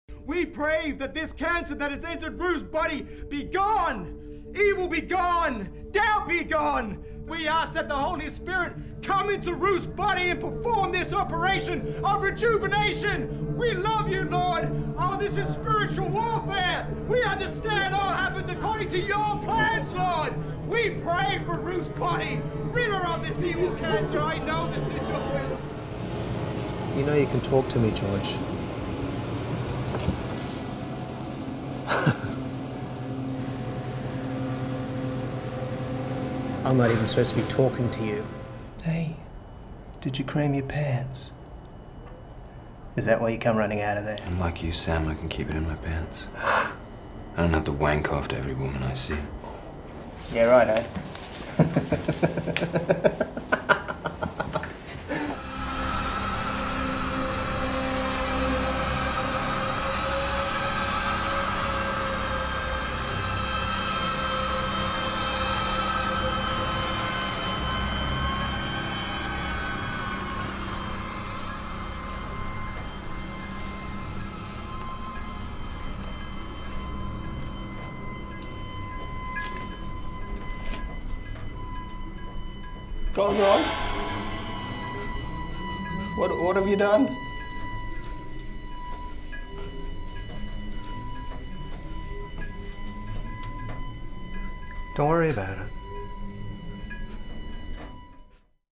Will Kelly Demo Reel 2020
Demo reel of Brisbane based actor Will Kelly's acting work. Will Kelly is a Brisbane based actor, speaker, producer and storyteller with over 20 years experience working in Short Films, Ads, TV & Theatre.